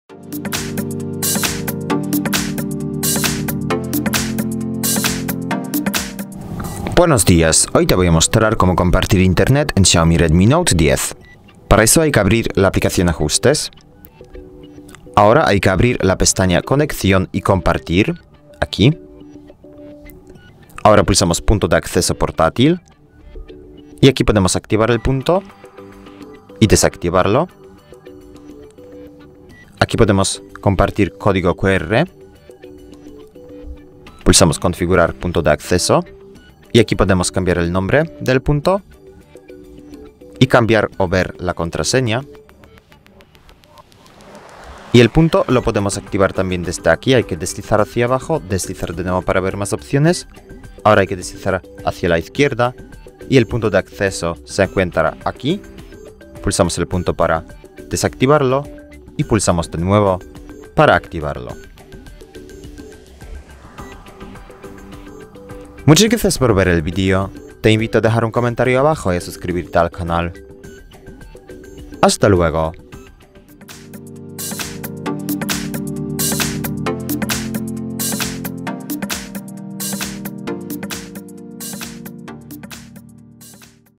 Cómo compartir Internet en XIAOMI Redmi Note 10 - activar HotSpot
En este tutorial te mostramos cómo activar punto de acceso WiFi en XIAOMI Redmi Note 10. Si necesitas compartir conexión de red con otro celular, quédate con nosotros y descubre cómo configurar HotSpot y compartir Internet en tu XIAOMI Redmi Note 10.